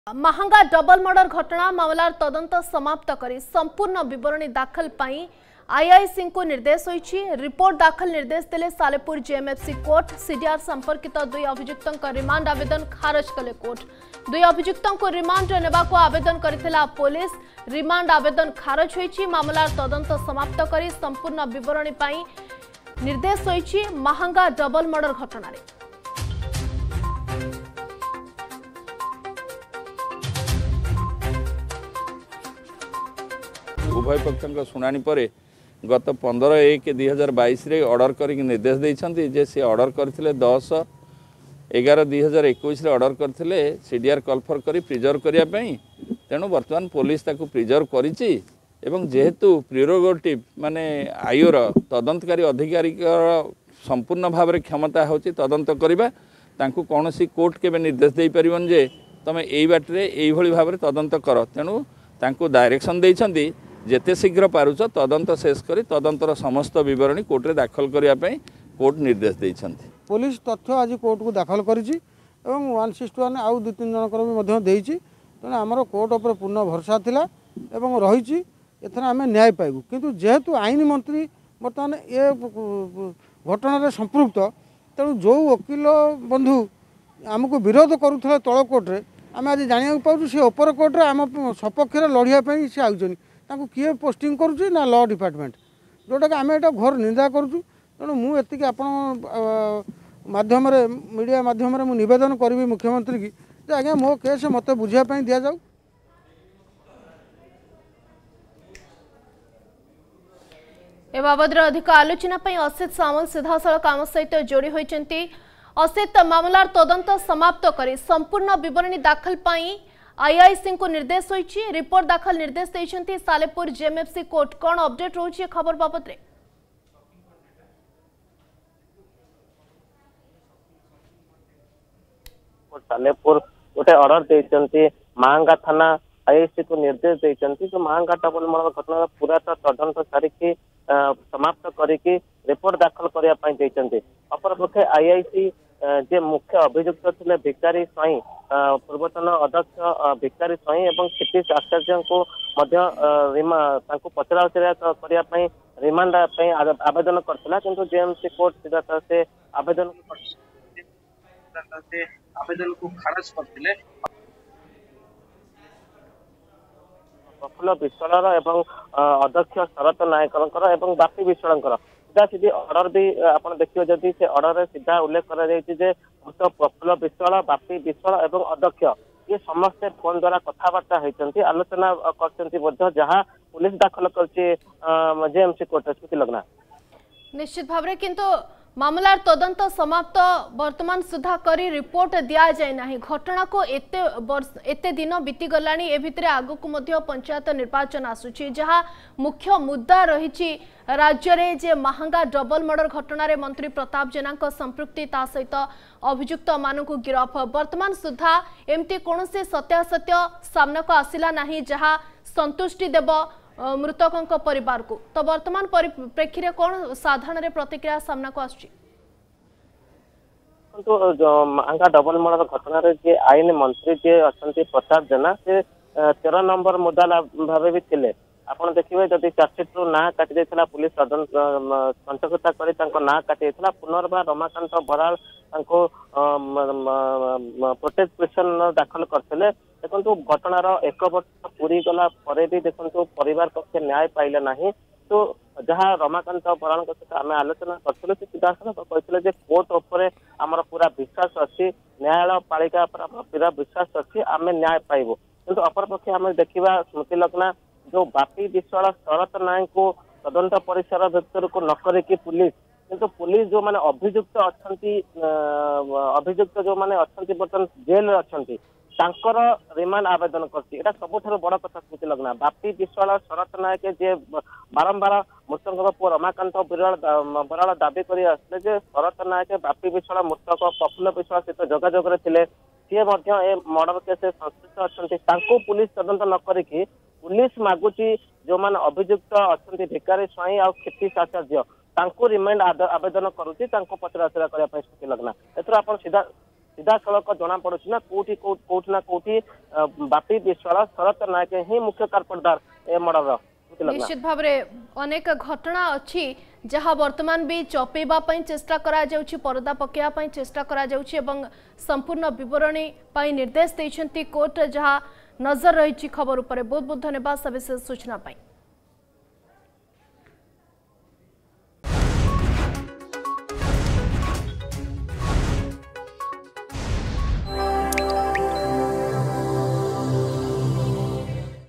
ମାହାଙ୍ଗା ଡବଲ ମର୍ଡର ଘଟଣା
ମାହାଙ୍ଗା ଡବଲ ମର୍ଡର ଘଟଣା । ମାମଲାର ତଦନ୍ତ ସମାପ୍ତ କରି ସମ୍ପୂର୍ଣ୍ଣ ବିବରଣୀ ଦାଖଲ ପାଇଁ ଆଇ ଆଇଆଇସିଙ୍କୁ ନିର୍ଦ୍ଦେଶ । ରିପୋର୍ଟ ଦାଖଲ ନିର୍ଦ୍ଦେଶ ଦେଲେ ସାଲେପୁର JMFC କୋର୍ଟ । CDR ସମ୍ପର୍କିତ 2 ଅଭିଯୁକ୍ତଙ୍କ ରିମାଣ୍ଡ ଆବେଦନ ଖାରଜ କଲେ କୋର୍ଟ । 2 ଅଭିଯୁକ୍ତଙ୍କୁ ରିମାଣ୍ଡରେ ନେବାକୁ ଅବେଦନ କରିଥିଲା ପୋଲିସ ।